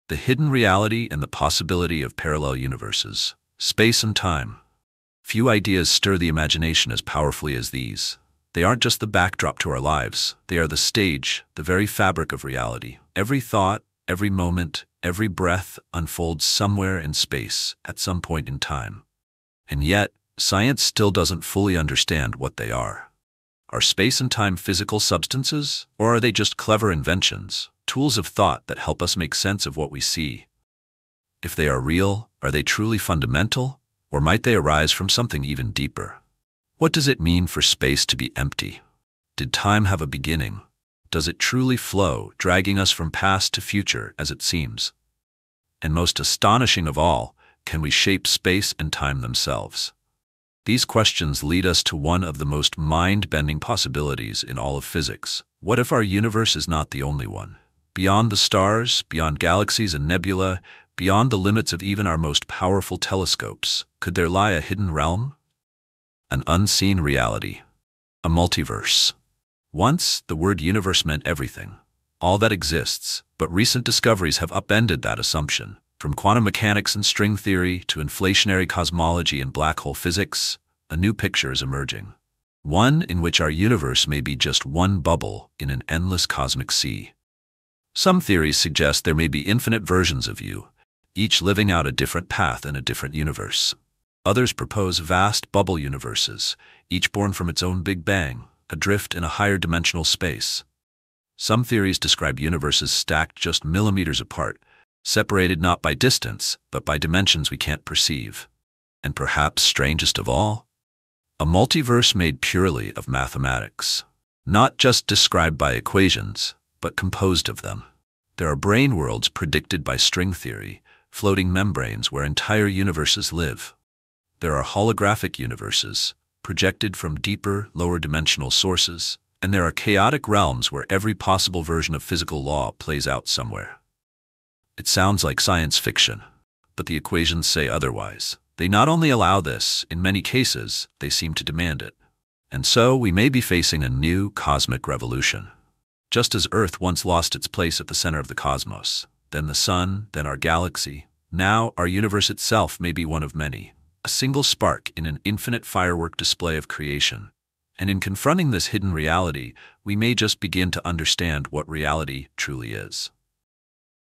Multiverse space universe facts
There exists a possibility of parallel universes. An unseen reality of a multiverse. Could there be infinite versions of each of us? Or maybe we live in a bubble...